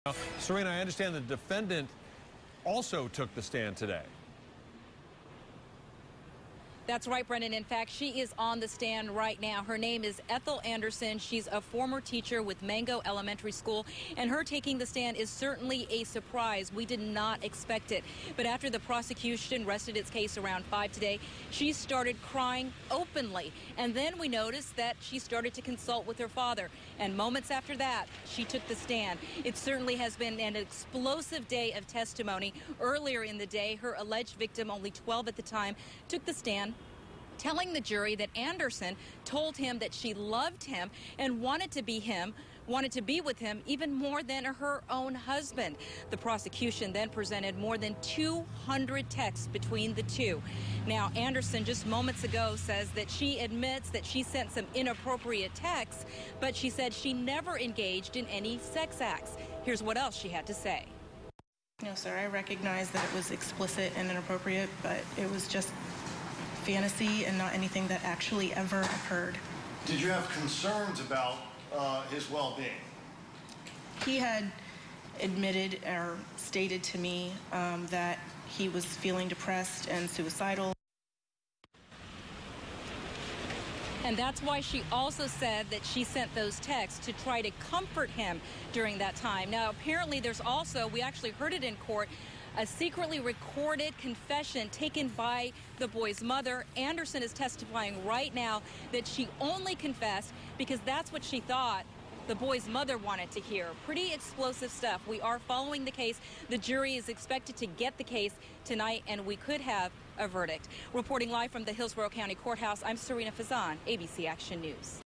Teacher in molestation case says student was depressed
A boy who turned 14 today took the stand and testified against former teacher.